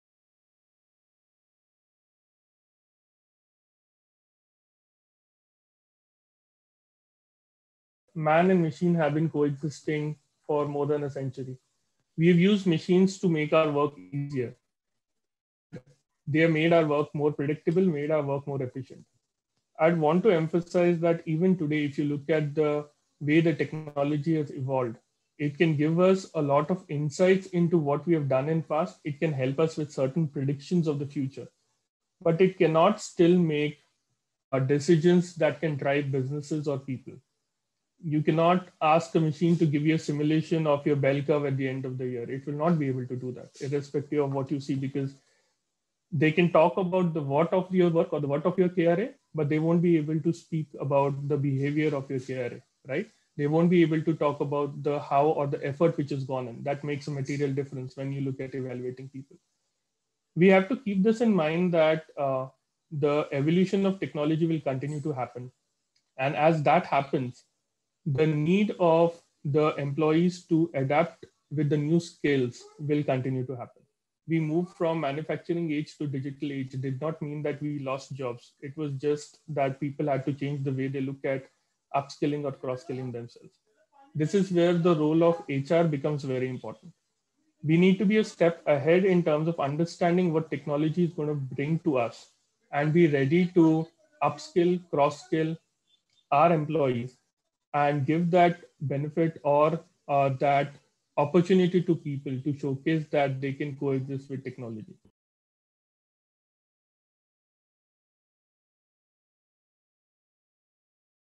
Technology cannot still make decisions that can drive businesses or people - Navjot Dhar
The evolution of technology will continue to happen and as that happens, the need of the employees to adapt with the new skills will continue to happen.
Navjot Dhar, Head Human Resources, IT and Business Strategy, Tata SmartFoodz shares his perspective on the evolving role of HR as the future as we moved from manufacturing age to digital age.